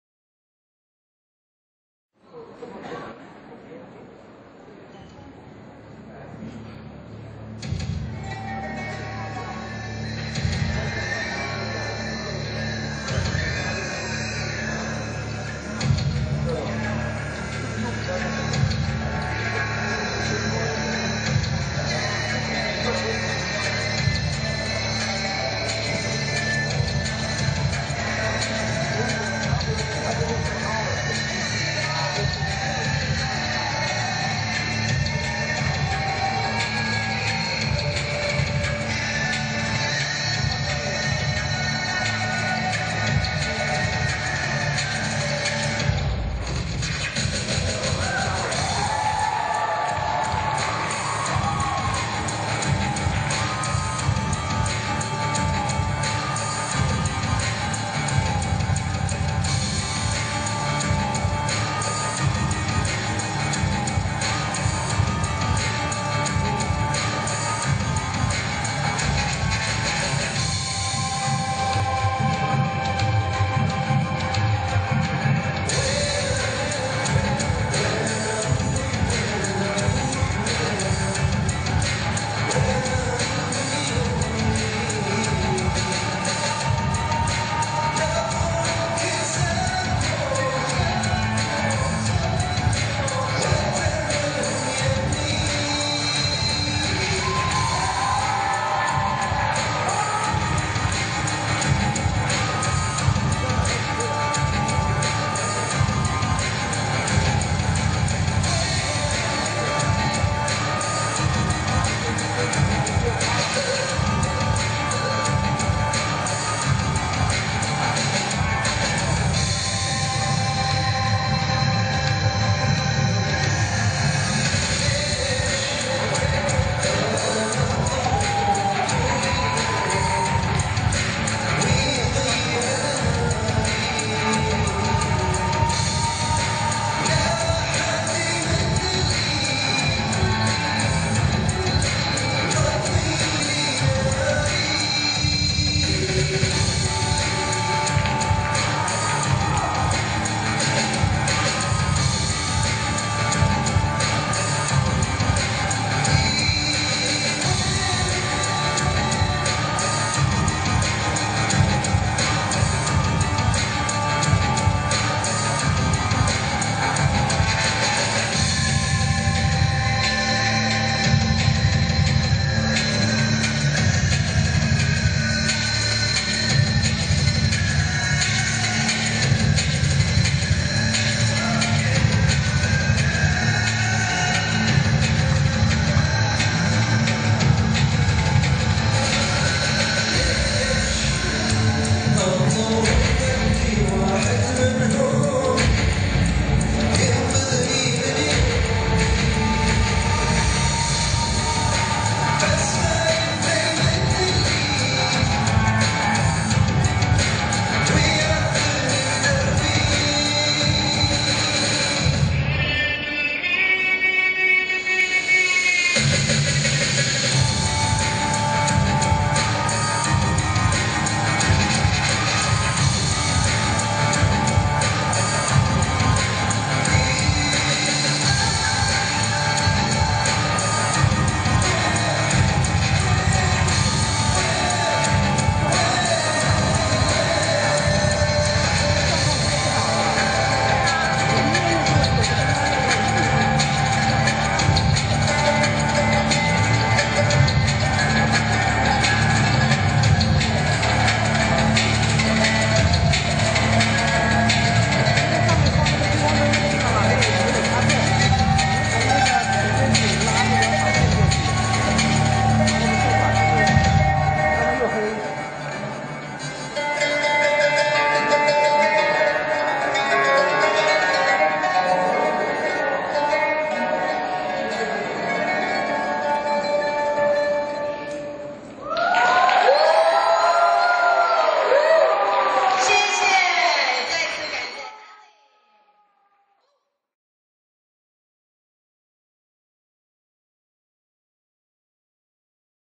Chinese Sword fusion belly dance
The Sword dance
The sword dance is a particular form of folk dance in the Han and the Tang dynasties. The sword is used mainly for the movement of the dance and less as a weapon. A single, double or dagger sword can be used to perform this kind of dance. A single sword has hanging threats for ornamental purpose. It can extend to the length of one meter. The tassels add to the splendour of the dance. The movement of the sword dance bears a heroic expression with a number of variations. The movement of the dance is also elegant with beauty and grace.